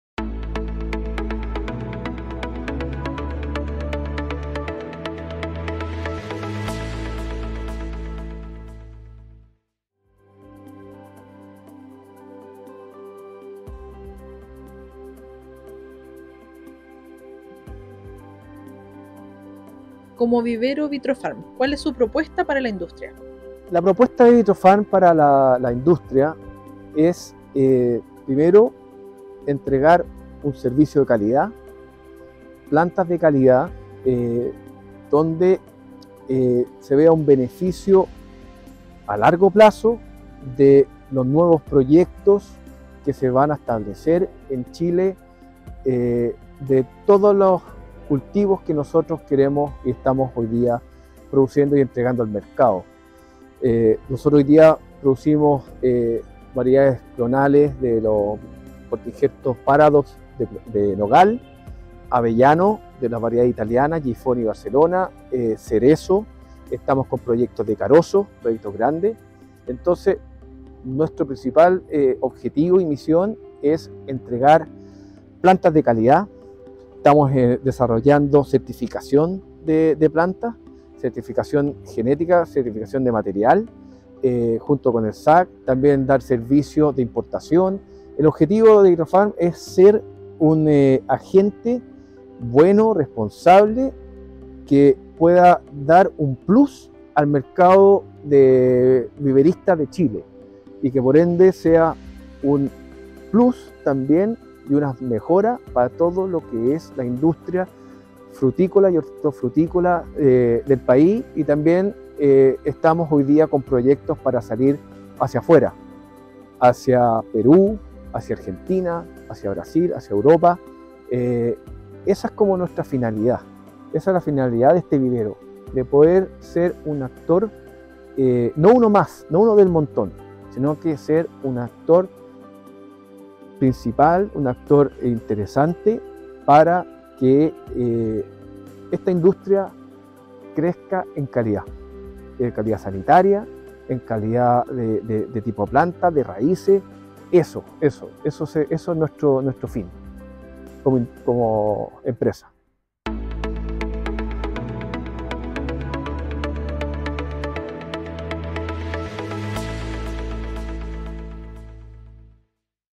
La propuesta diferenciadora de Vitrofarm para la industria
¡Descubre la propuesta diferenciadora de Vitrofarm para la industria de la mano de su socio director, Nicolás Manterola!